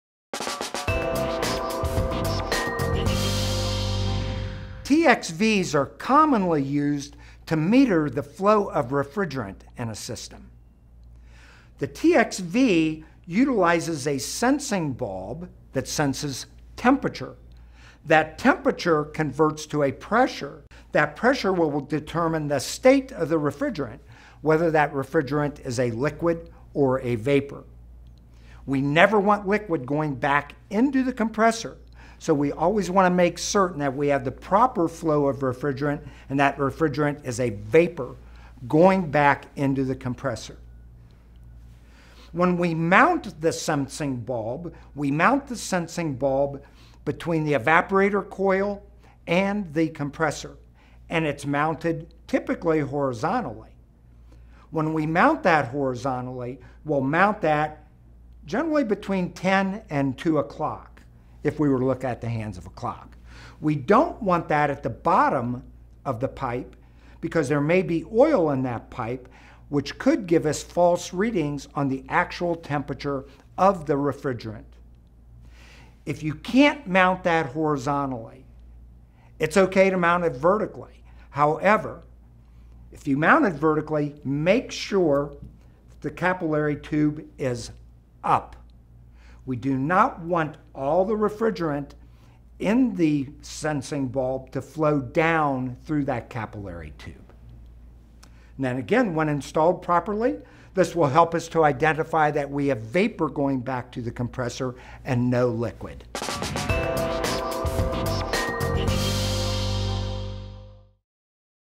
Mounting TVX's- What You Need To Know
Why is it preferred that a TXV is mounted horizontally?  If mounted vertically why must it be mounted in a specific orientation?  Packard gives you the knowledge you need for success on the job.  For more information on Packard Academy training for technicians, please visit our Packard supplies to Wholesalers only.